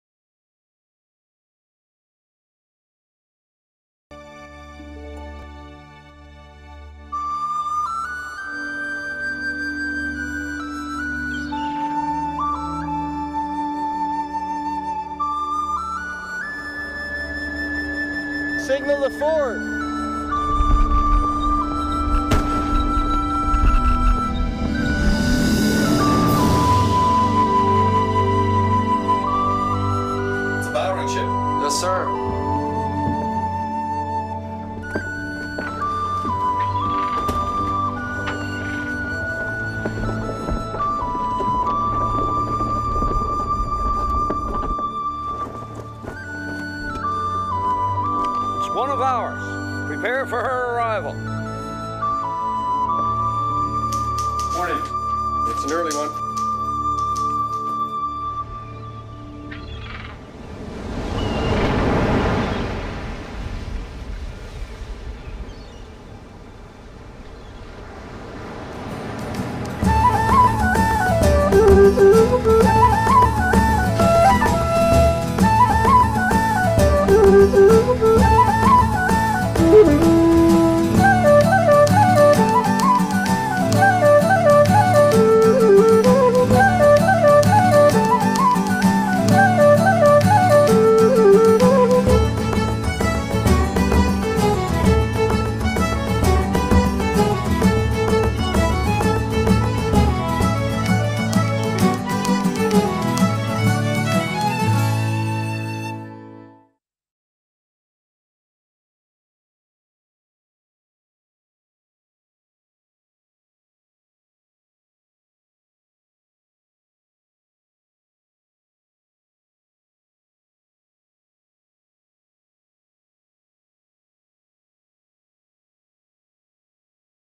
The Hill - Trailer
Relive the History at the Signal Hill National Historic Site Visitor Centre Exhibit.